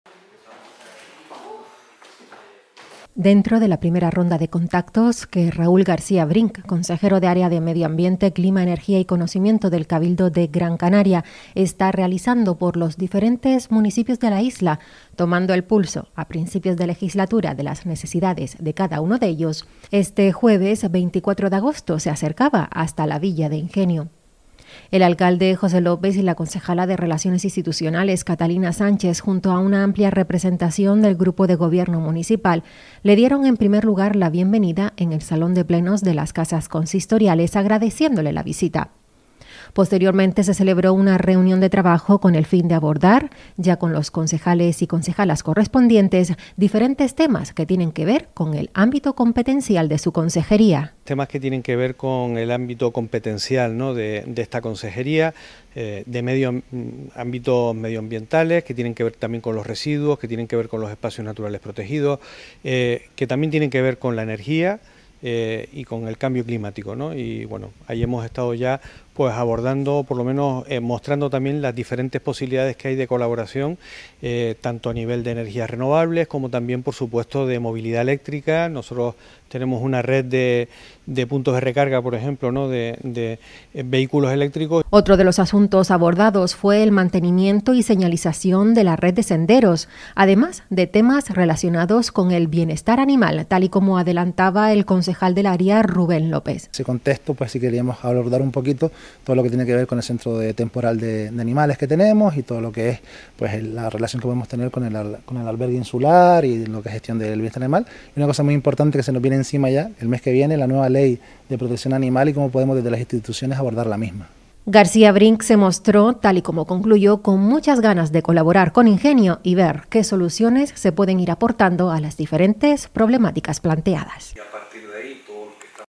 Primera visita institucional del consejero insular de Medio Ambiente, Clima, Energía y Conocimiento
Los espacios naturales protegidos, la red de senderos, energías renovables, puntos de recarga y bienestar animal fueron solo algunos de los temas abordados

Dentro de la primera ronda de contactos que Raúl García Brink, consejero de Área de Medio Ambiente, Clima, Energía y Conocimiento del Cabildo de Gran Canaria, está realizando por los diferentes municipios de la isla, tomando el pulso, a principios de legislatura, de las necesidades de cada uno de ellos, este jueves, 24 de agosto, se acercaba hasta la Villa de Ingenio.

El alcalde, José López, y la concejala de Relaciones Institucionales, Catalina Sánchez, junto a una amplia representación del grupo de gobierno municipal, le dieron, en primer lugar, la bienvenida en el Salón de Plenos de las Casas Consistoriales, agradeciéndole la visita. Posteriormente se celebró una reunión de trabajo con el fin de abordar, ya con los concejales y concejalas correspondientes, diferentes temas que tienen que ver con el ámbito competencial de su consejería, "en especial temas del ámbito medioambiental, que tienen que ver con los residuos, con los espacios naturales protegidos, con la energía y con el cambio climático. También queremos mostrar todas las posibilidades que hay de colaboración tanto a nivel de programas de instalación de energías renovables como de movilidad eléctrica, como los puntos de recarga de vehículos eléctricos, por ejemplo," explicó el consejero insular.

Otro de los asuntos abordados fue el mantenimiento y señalización de la red de senderos, además de temas relacionados con el bienestar animal, tal y como adelantaba el concejal del área, Rubén López. "En ese contexto abordamos lo que tiene que ver con el Centro de Estancia Temporal de Animales (CETA) que tenemos y la relación que podemos tener con el albergue insular y la gestión del bienestar animal, además de un tema muy importante y que se nos viene ya encima el mes que viene, la nueva Ley de Protección Animal y cómo podemos desde las instituciones abordar la misma," informó.

García Brink se mostró, tal y como concluyó, "con muchas ganas de colaborar con Ingenio y ver qué soluciones se pueden ir aportando a las diferentes problemáticas planteadas".